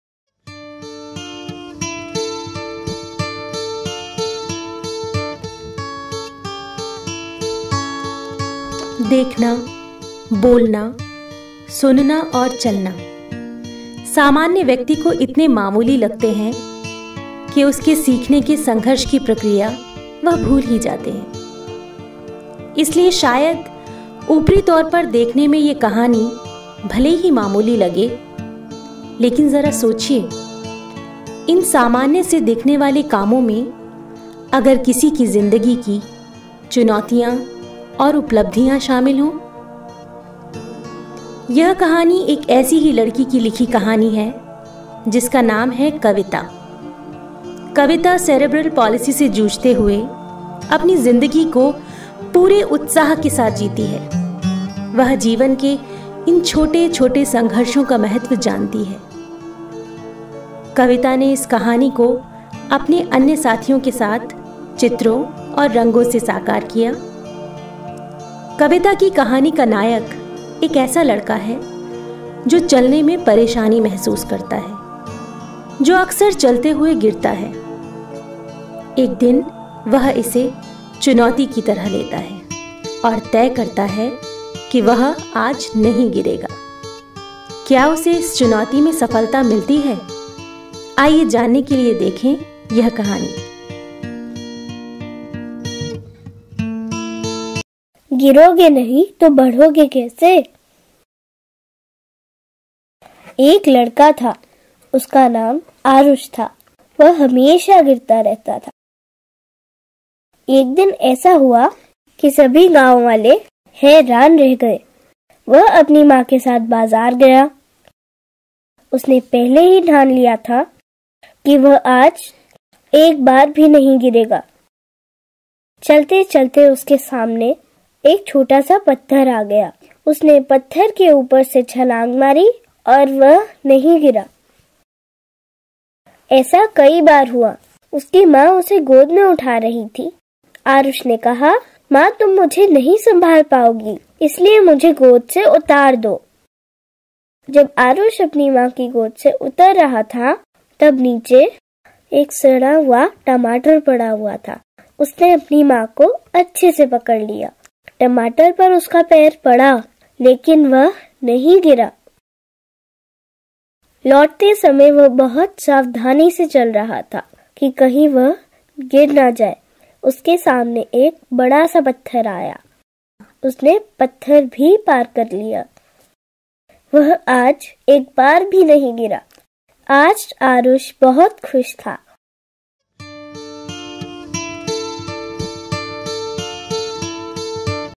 Giroge Nahi to badhoge kase
This video is an ICT enabled audio-visual story appealing for both children and adults.It is an effort to create children’s own stories in digital formate.